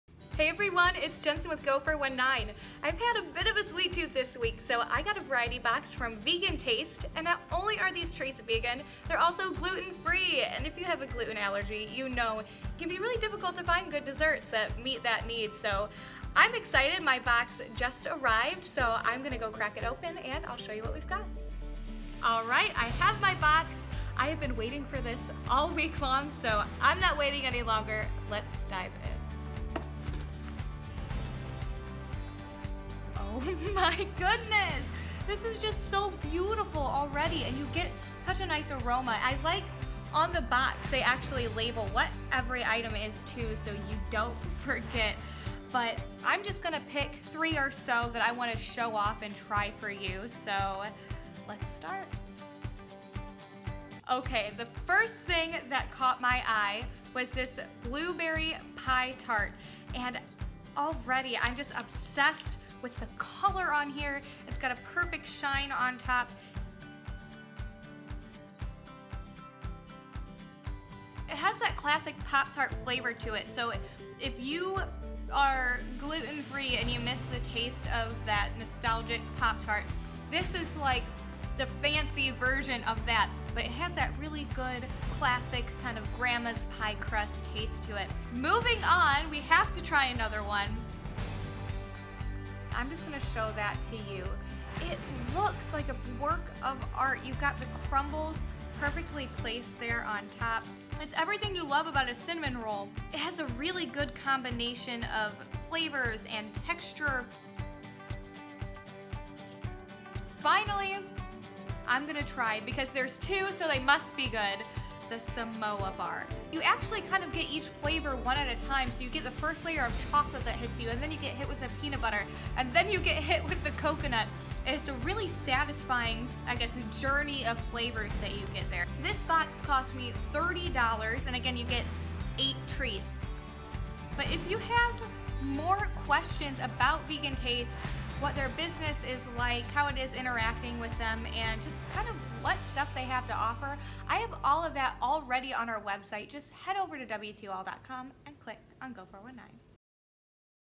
Vegan Taste brings inclusivity to classic confections | Go 419 Takeout Takeover
Classic treats with a modern twist, made from ingredients that almost everyone can enjoy.